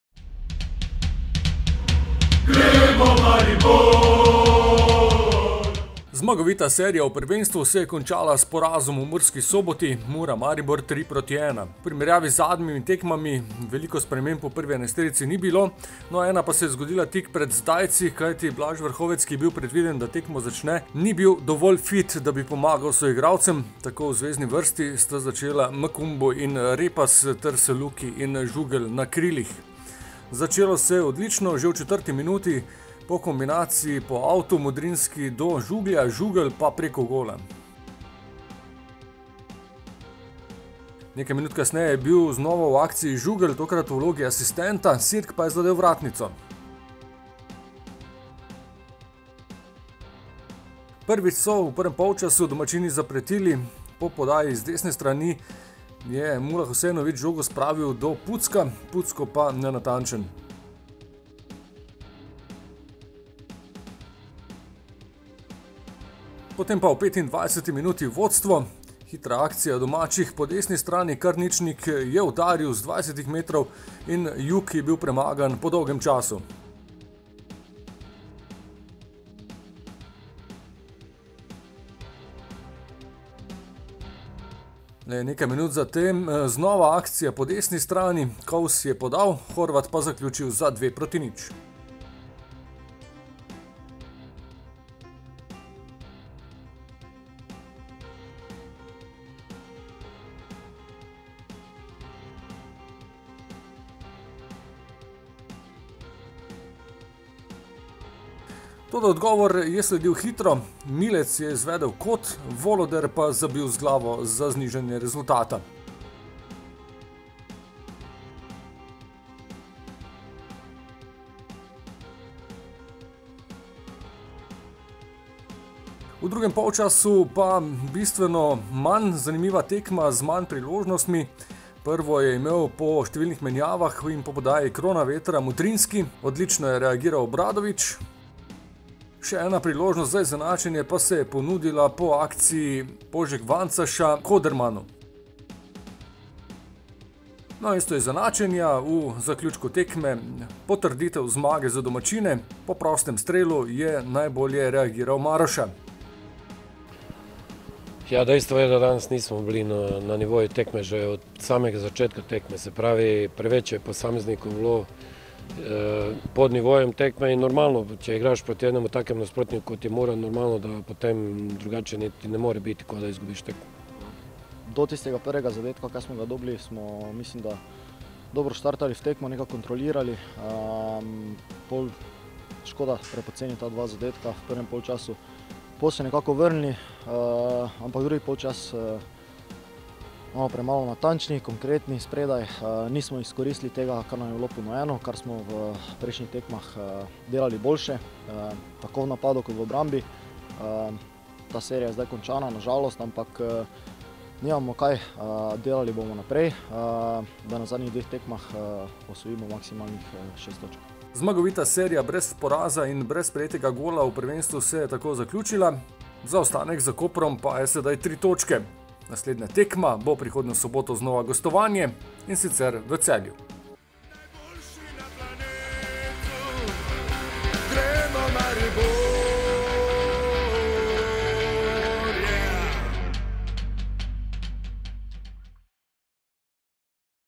MURA - MARIBOR 3:1 (2:1)
Od Mure do Mure je trajala vijol’čna prvenstvena zgodba o neporaženosti. Po osmih tekmah, ko smo povrnili stabilnost moštva, z igrišča praznih rok. Prvi prvenstveni poraz moštva, odkar je na klopi Radovan Karanović.

Vratar Ažbe Jug je po 568 minutah moral po žogo v mrežo, skupaj trikrat. Vmes je Robert Voloder vrnil upanje z zmanjšanjem zaostanka na 1:2, toda poti do izenačenja ali preobrata nismo našli. Po petih zaporednih zmagah v ligaškem tekmovanju konec uspešne serije, do konca jesenskega dela pa še 2 tekmi. Najprej v soboto v Celju.